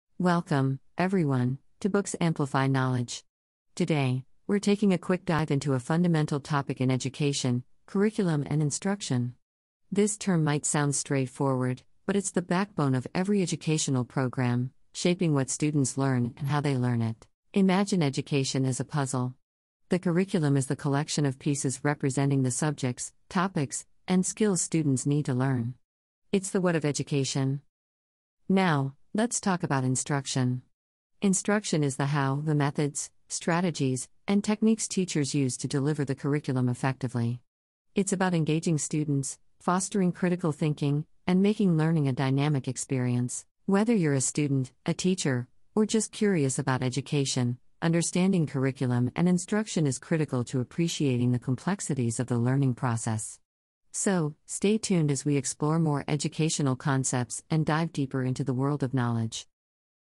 Mastering Curriculum and Instruction | Unlock Your Teaching Potential
"Unlock the secrets of effective teaching with our comprehensive guide on Curriculum and Instruction. This video will provide the tools and strategies to design impactful lessons, foster a positive learning environment, and maximize student engagement. Whether you're a seasoned or novice educator, this video is your key to unlocking your teaching potential. Tune in to transform your teaching methods and make learning exciting for your students."